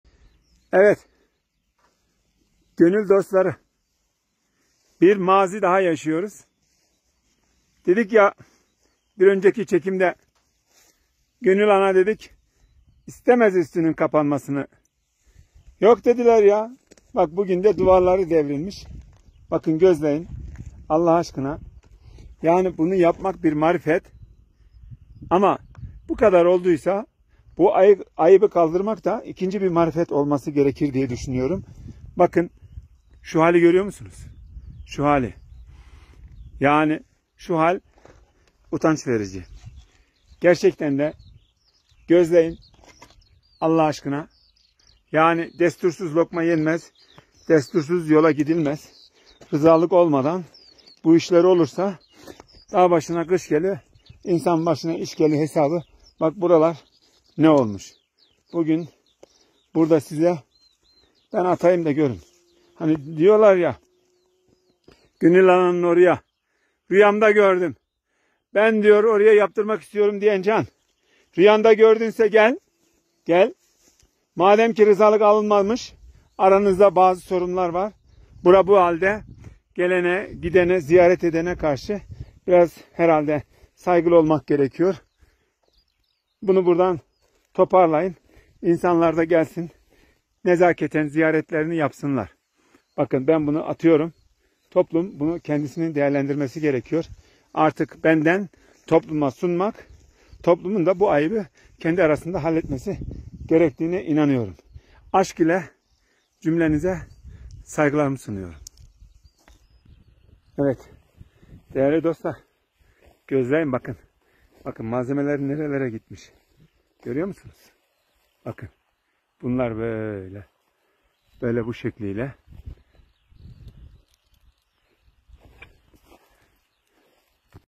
‘Rüyamda gördüm’ diyerek inşaat başlattılar; Gönül Ana’nın mezarını ziyaret edemiyoruz'
PİRHA - Hubyar Köyü’ne bağlı Tekeli Dağı’ndaki Gönül Ana’nın mezarı göz göre göre tahrip edildi. Dede Erdoğan Sezer, bir kişinin, söz konusu mezarlığı rüyasında görüp üzerini kapatmak için inşaat başlattığını, ancak civardaki insanların buna rızalık vermediğini aktardı. Sezer, “Şimdi türbeye varıp niyaz dahi edemiyoruz. Bu durum beni çok üzdü” diye de belirtti.